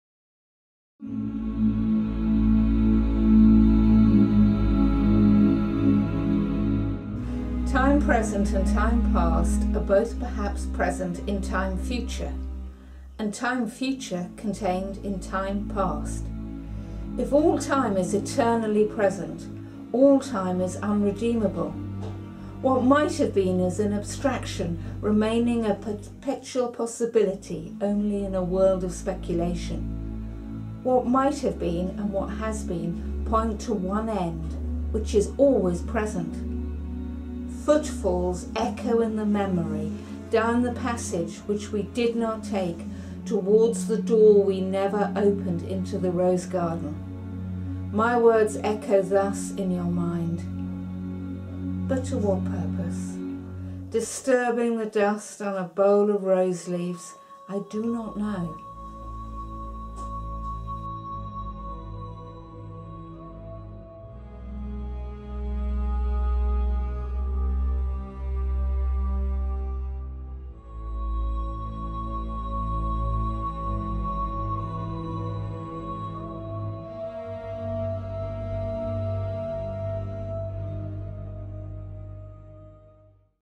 Burnt Norton, Four Quartets, T S Eliot
Victoria Harvey reads the beginning of the poem